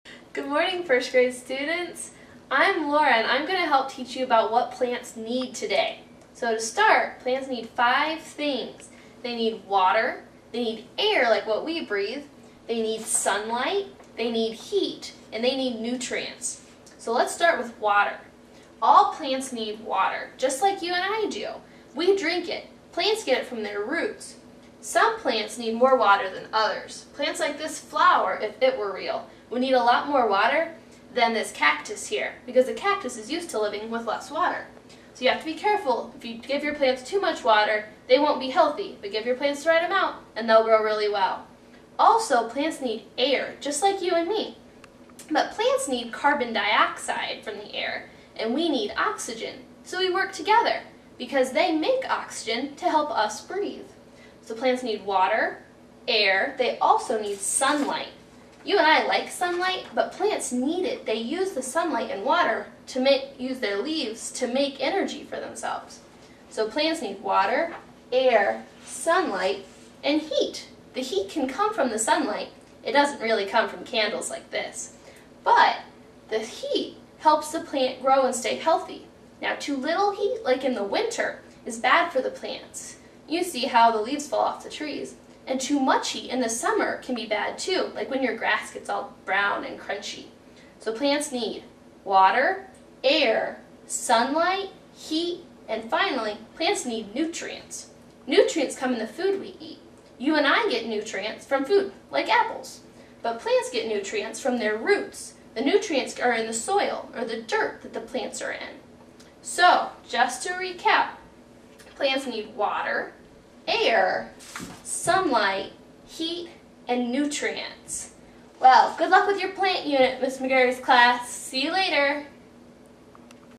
What do plants need?
Made for my Honors w200 class at IU, spring semester 2010.  A first grade lesson on what plants need.